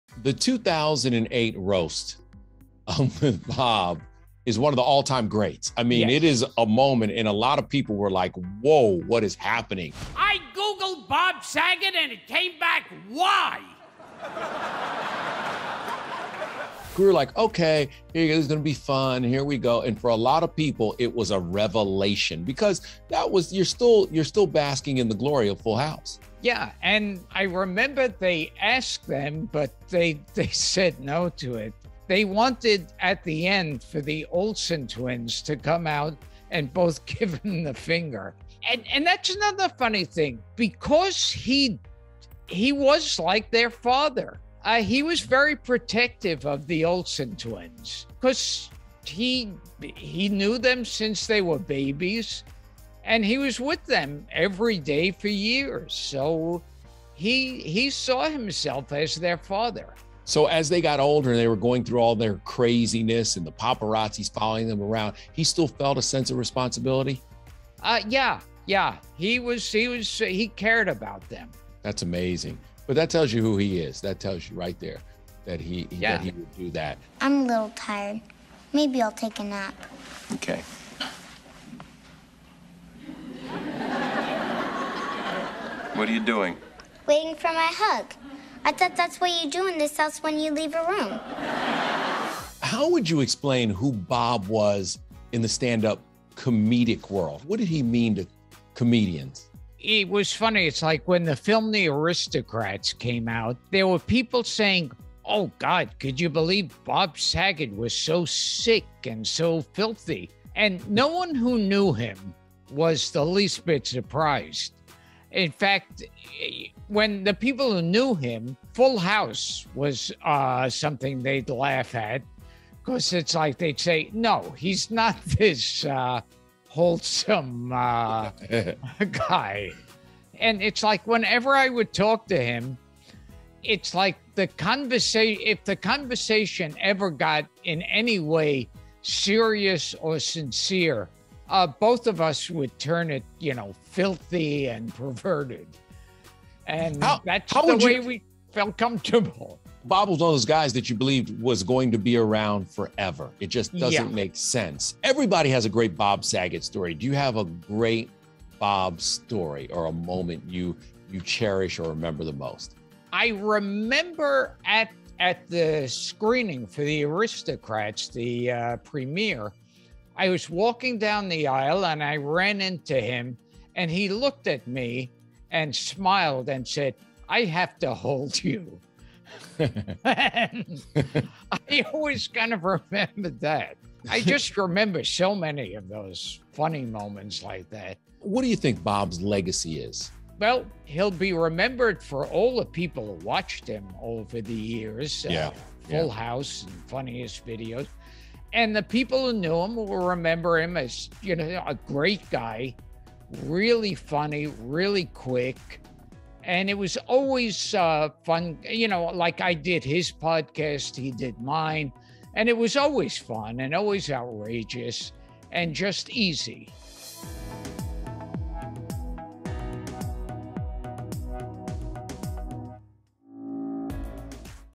Bob Saget Was PROTECTIVE of Mary-Kate Olsen and Ashley Olsen, Says Gilbert Gottfried (Exclusive)
Gilbert Gottfried is reflecting on his friendship with the late Bob Saget. ET's Kevin Frazier spoke with the 66-year-old comedian about Saget's unexpected death on Sunday and he shared what the 'Full House' star means to the comedy world. Gottfried also explained why Saget felt a paternal connection to Mary-Kate and Ashley Olsen, who shared the role of Michelle Tanner, Saget’s on-screen daughter, on 'Full House.'